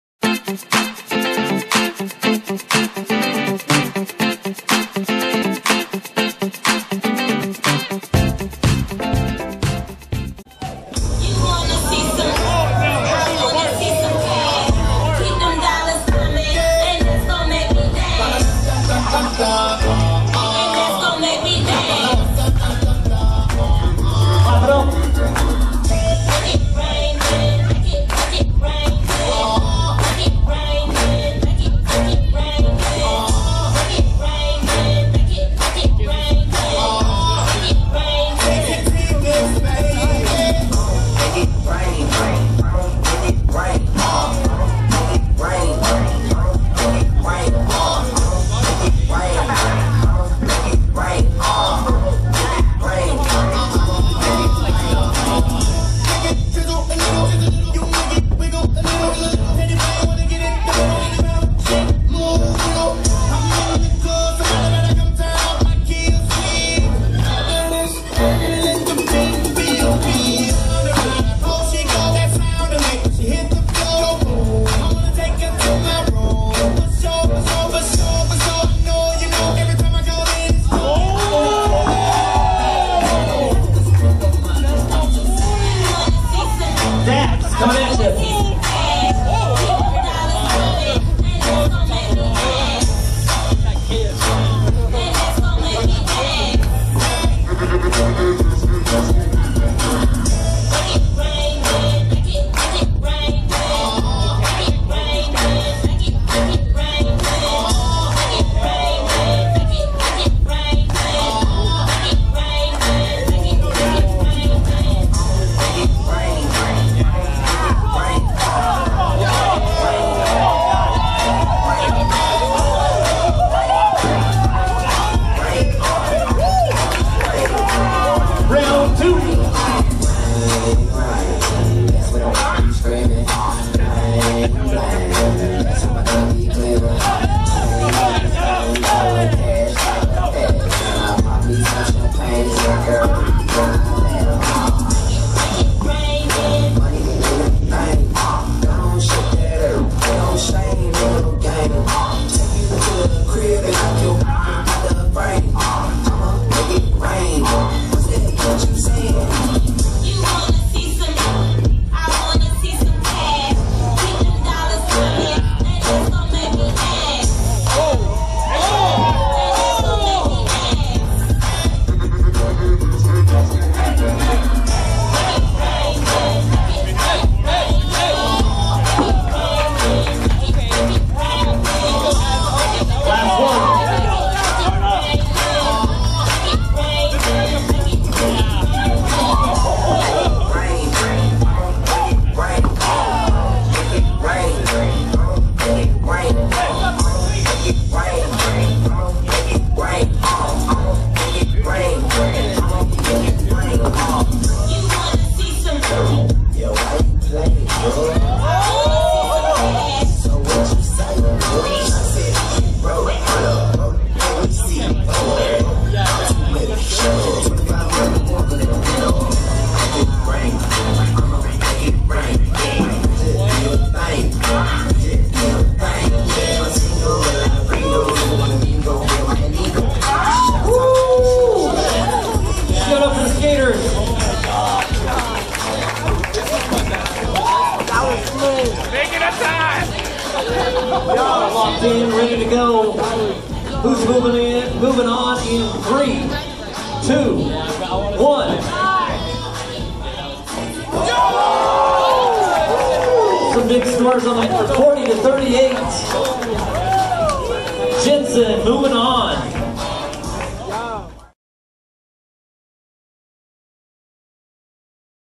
Jensen vs Dax Mens Solo Battles
Mens Solo Battles

Jensen vs Dax

Summer Jam 2019
August 7-10
Classic Fun Center
Sandy, Utah

Jensen wins
Judges Scores:
Jensen 40
Dax 38